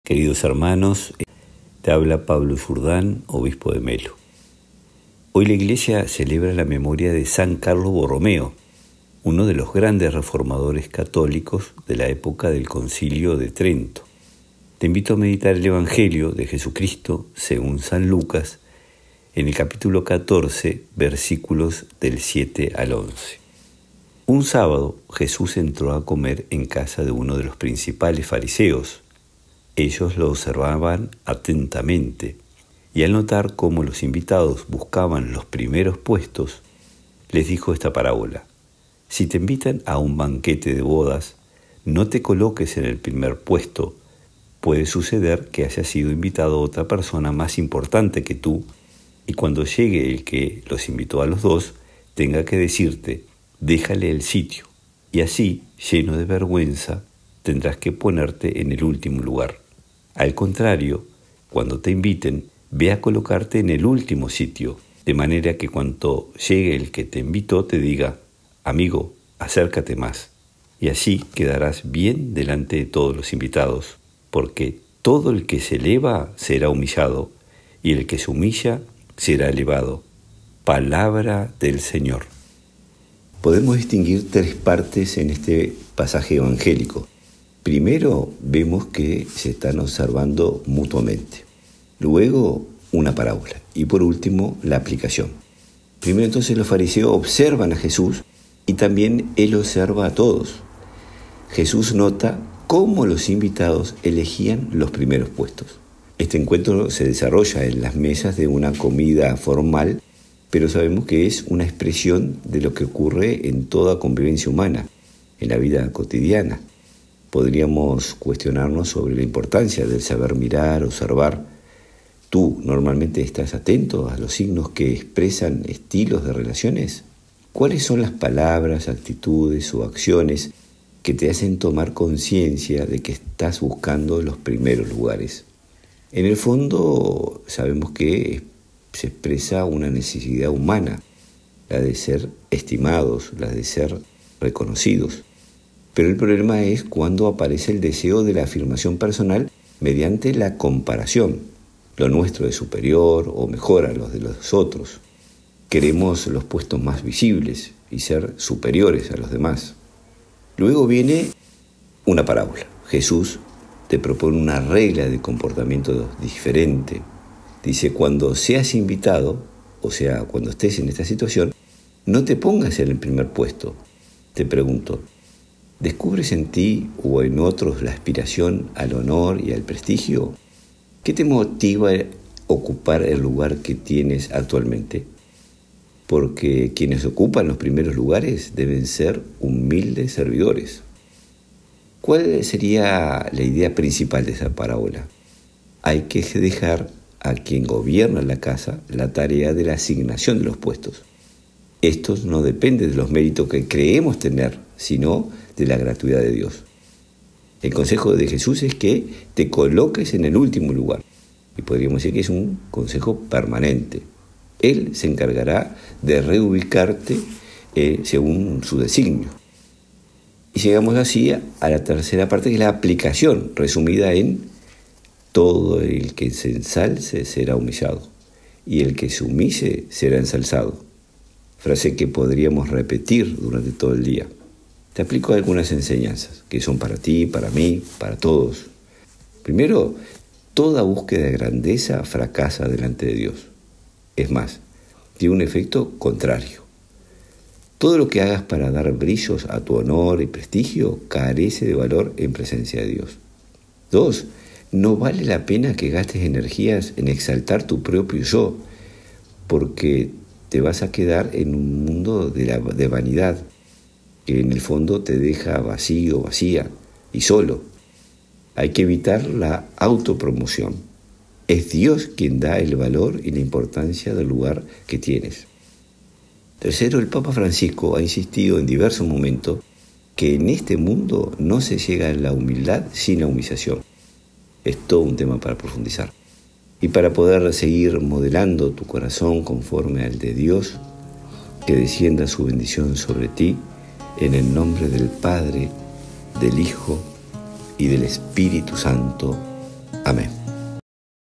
Evangelio del Día 04/11/2023, según San Lucas 14.1.7-11 - Mons. Pablo Jourdan
SAN CARLOS BORROMEO, OBISPO

Evangelio según San Lucas 14,1.7-11

Un sábado, Jesús entró a comer en casa de uno de los principales fariseos. Ellos lo observaban atentamente. Y al notar cómo los invitados buscaban los primeros puestos, les dijo esta parábola:

"Si te invitan a un banquete de bodas, no te coloques en el primer puesto, puede suceder que haya sido invitada otra persona más importante que tú, y cuando llegue el que los invitó a los dos, tenga que decirte: 'Déjale el sitio', y así, lleno de vergüenza, tendrás que ponerte en el último lugar.

Al contrario, cuando te inviten, ve a colocarte en el último sitio, de manera que cuando llegue el que te invitó, te diga: 'Amigo, acércate más', y así quedarás bien delante de todos los invitados. Porque todo el que se eleva será humillado, y el que se humilla será elevado".

Reflexión de Mons. Pablo Jourdan (Obispo de Melo, Uruguay)